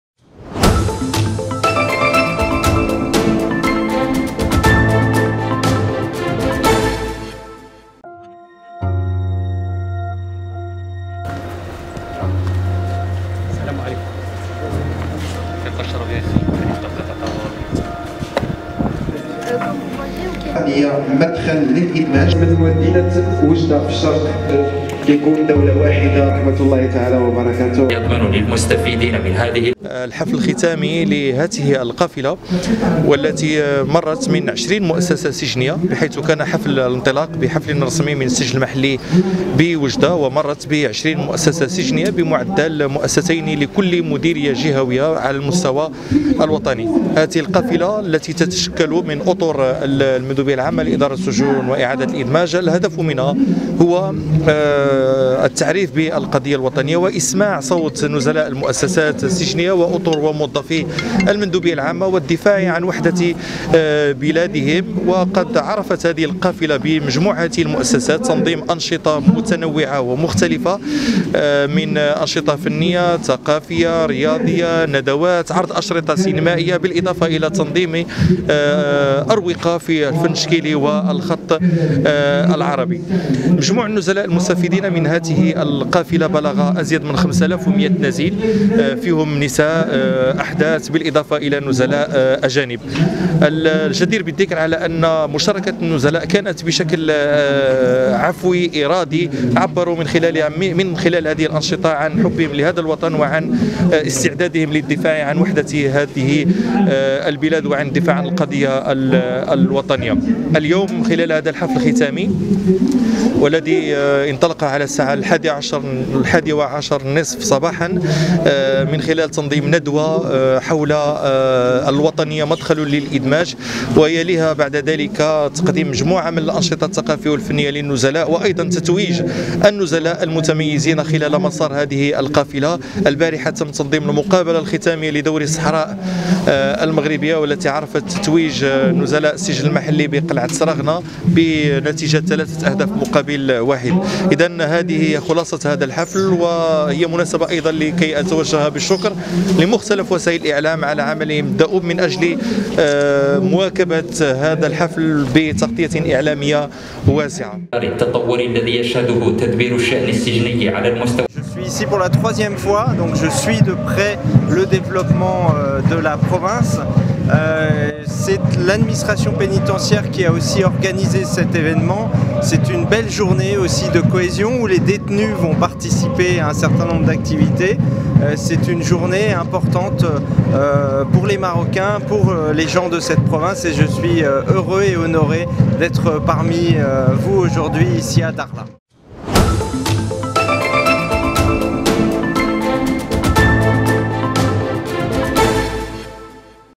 أكثر من 5 آلاف نزيل شاركوا في قافلة الصحراء المغربية بالسجون
حطت القافلة الوطنية للصحراء المغربية بالمؤسسات السجنية الرحال بالسجن المحلي بالداخلة، يوم 6 نونبر 2021،
وكانت الانطلاقة الرسمية لهذه القافلة قد أعطيت من السجن المحلي وجدة 2 بتاريخ 31 ماي 2021 بحضور عدد من المسؤولين وفعاليات المجتمع المدني.
ومرت هذه القافلة عبر 20 مؤسسة سجنية على امتداد التراب الوطني، ليكون اختتام القافلة بالسجن المحلي بالداخلة. وشهدت كل محطة من المحطات المذكورة تنظيم عدد من الأنشطة والفقرات التي تتمحور حول التحسيس بأهمية قضية الصحراء المغربية بين صفوف نزلاء تلك المؤسسات.

# أخبار_عاجلة